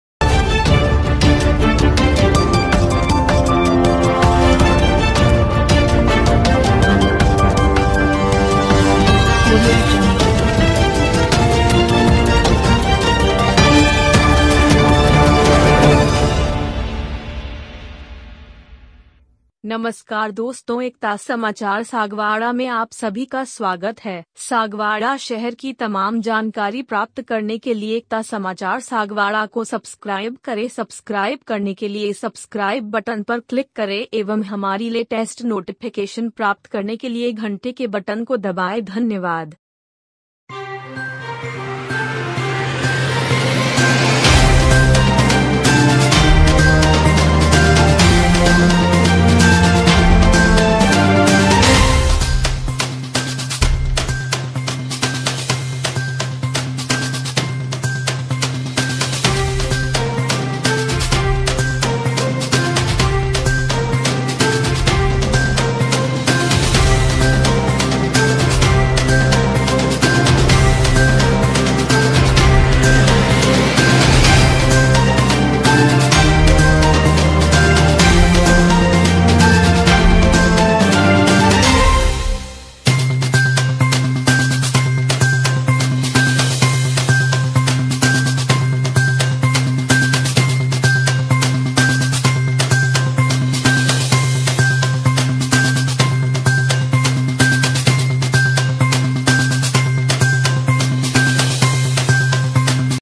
भारतीय जनता पार्टी का सदस्यता अभियान सागवाड़ा में हुआ
किसान मोर्चा जिला सह संयोजक प्रदीप कुमार गामोट,अल्पसंख्यक प्रदेशमंत्री शेख राजूभाई बर्तनवाला,st मोर्चा के जयंतिलालजी कटारा ने बनाये एक ही दिन में 100 से ज्यादा सदस्य🙏प्रदीप गामोट ने बताया कि किसान मोर्चा में अभीतक 600 से ज्यादा सक्रिय सदस्य बन चुके है और ये प्रक्रिया आगे भी जारी रहेगी और दिए लक्ष्य को पूरा करने की पूरी कोशिश की जाएगी🙏